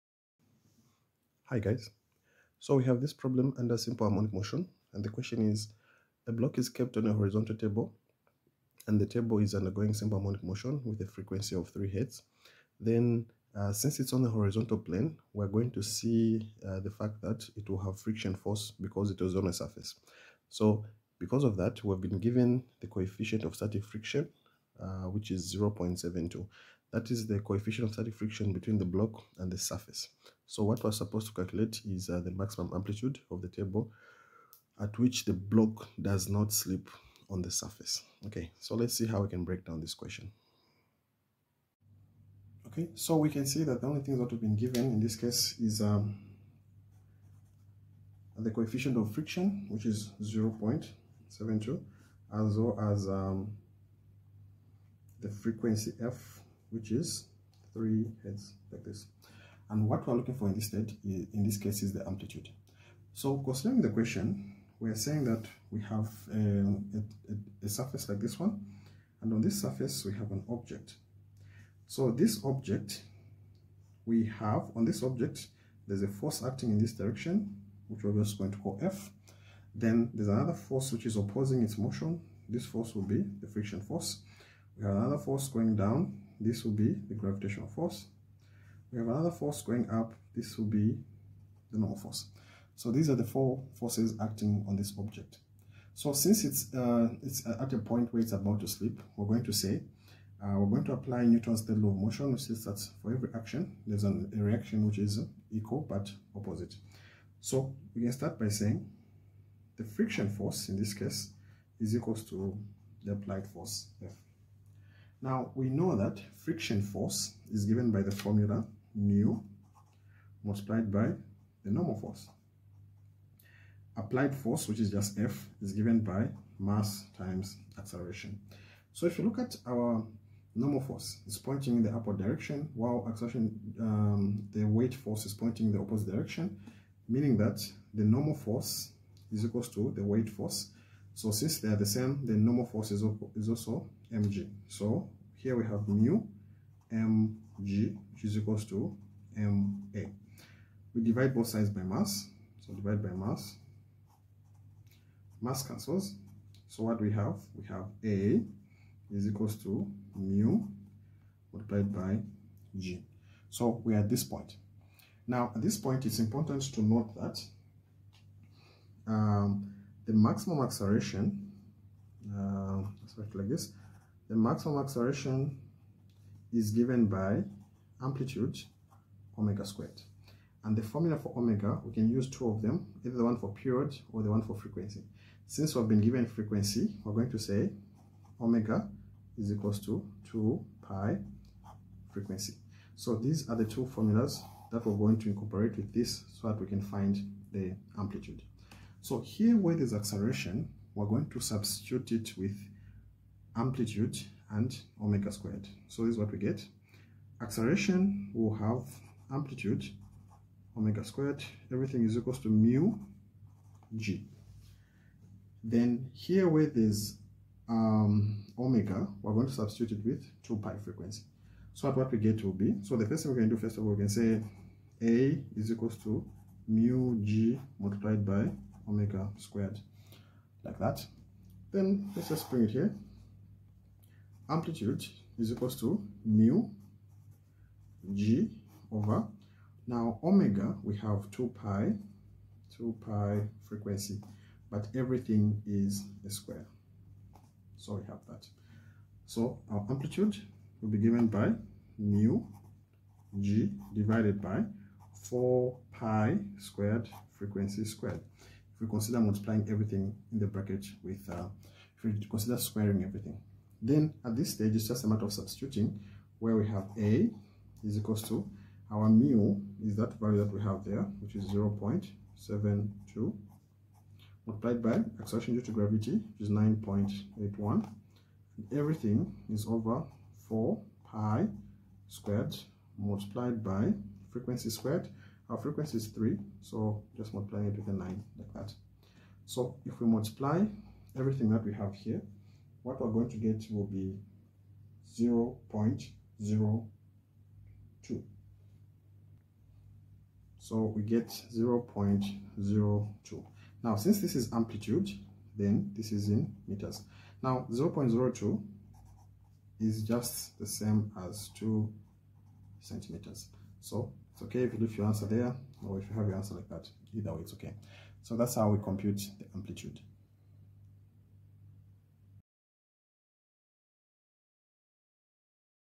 How to calculate Amplitude when given friction & the coefficient of static friction | S.H.M
A block is kept on a horizontal table. The table is undergoing simple harmonic motion of frequency 3 Hz in a horizontal plane. The coefficient of static friction between the block and the table surface is 0.72. Find the maximum amplitude of the table at which the block does not slip on the surface.
In mechanics and physics, simple harmonic motion (sometimes abbreviated SHM) is a special type of periodic motion an object experiences due to a restoring force whose magnitude is directly proportional to the distance of the object from an equilibrium position and acts towards the equilibrium position. It results in an oscillation that is described by a sinusoid which continues indefinitely (if uninhibited by friction or any other dissipation of energy).
Simple harmonic motion can serve as a mathematical model for a variety of motions, but is typified by the oscillation of a mass on a spring when it is subject to the linear elastic restoring force given by Hooke's law. The motion is sinusoidal in time and demonstrates a single resonant frequency. Other phenomena can be modeled by simple harmonic motion, including the motion of a simple pendulum, although for it to be an accurate model, the net force on the object at the end of the pendulum must be proportional to the displacement (and even so, it is only a good approximation when the angle of the swing is small; see small-angle approximation). Simple harmonic motion can also be used to model molecular vibration.
Simple harmonic motion provides a basis for the characterization of more complicated periodic motion through the techniques of Fourier analysis.